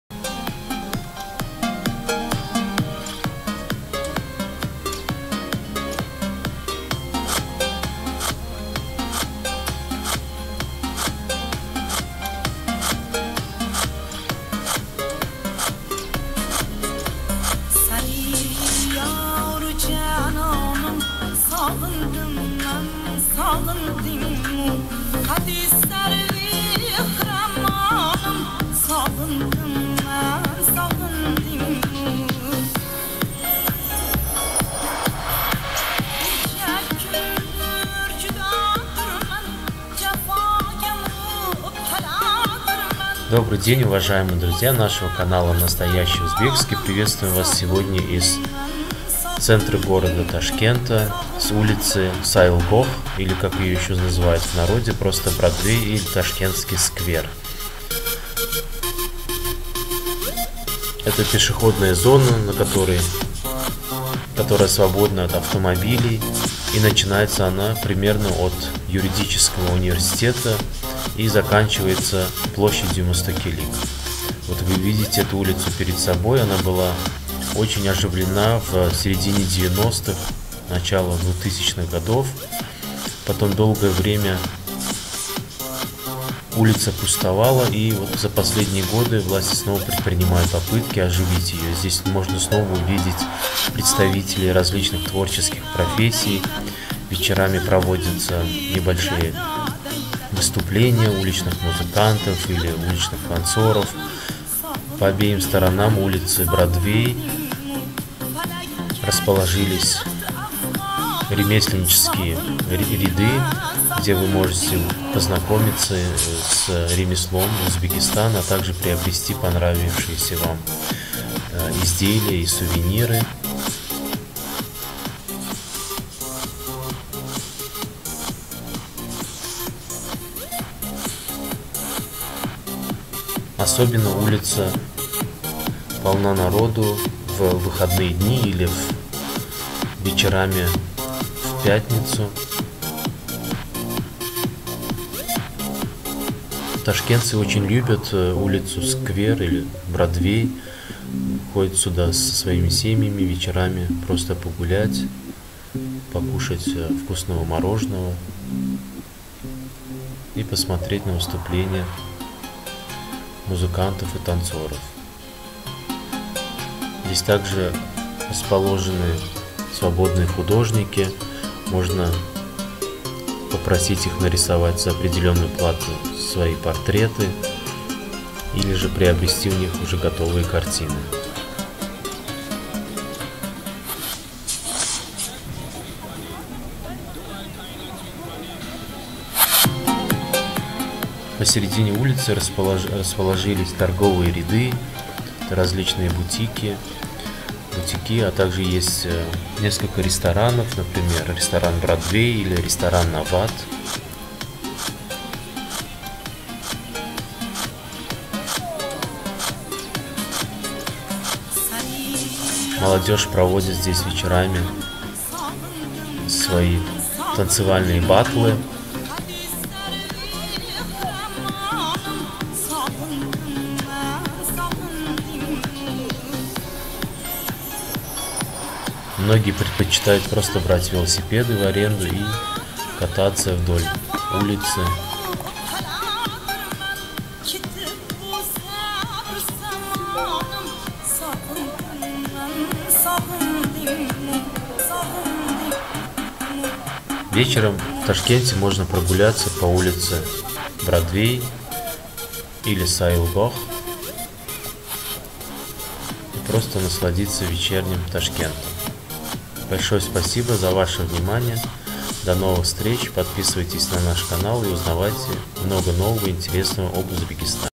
Бродвей, прогулочный сквер в Ташкенте, Сквер в Ташкенте
Бродвей, прогулочный Сквер в Ташкенте, сквер Амира Тимура в центре Ташкента был заложен в 1882 г. и носил первоначально название Константиновский сквер. За в истори своего суествования облик Сквера несеолько раз кардинально менялся. Сегодня это круглая парковая зона, от которой лучеобразно начинатся главные улицы Ташкента, одна из них Бродвей или Сайилгох, где вечерами собирается снова много народа, свободные музыканты, художники, филателисты.